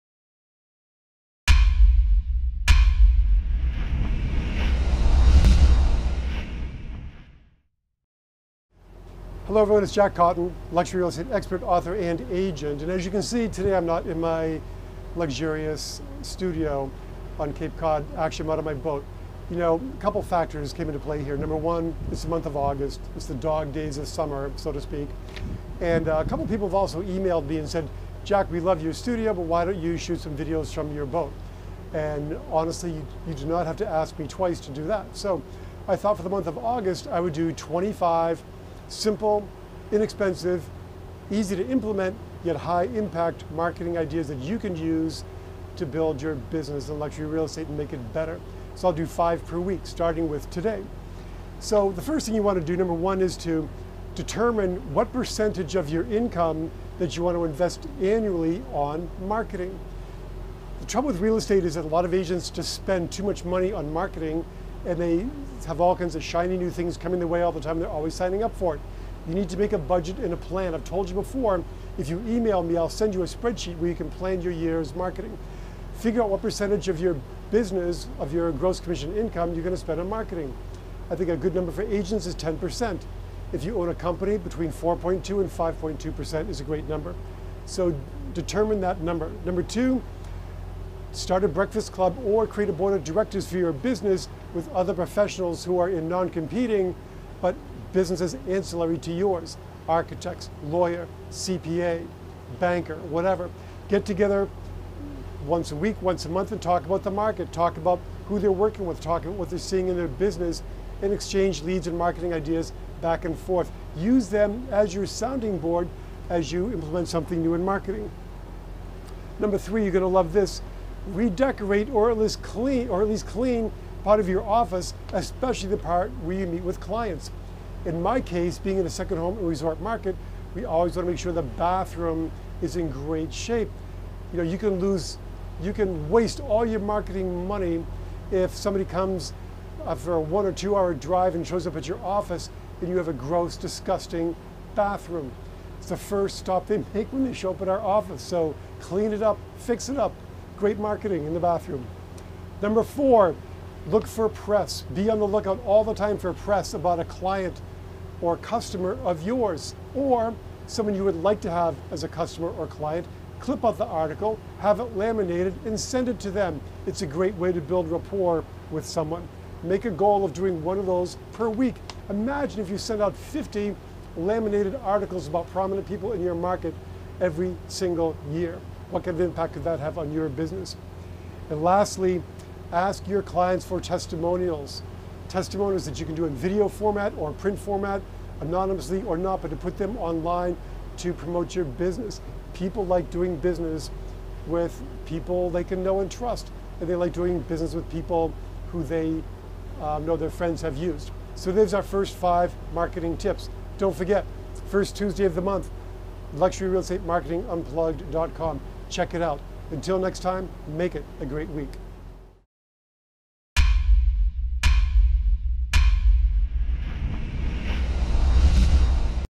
Are You Spending Too Much on Marketing?
- I’ve been asked to shoot some videos from the water and or from a boat this summer. You don’t have to ask me that twice!

Since it's August, I thought I would take a mellower approach and start a series of videos with 25 easy ways to implement low cost and high impact marketing ideas. Today, I will cover 5 ideas and do more each week. The first five are:

1. Determine a percentage of gross income to spend annually on marketing. Determine your industry average.
2. Join or organize a breakfast club or Personal Board of Directors with other professionals.
3. Redecorate or at least clean your office or location where you meet with your clients, paying special attention to one room in particular!
4. Laminate good press featuring a customer or client and send it along with a personal note.
5. Ask for and include "testimonials" from customers in your literature.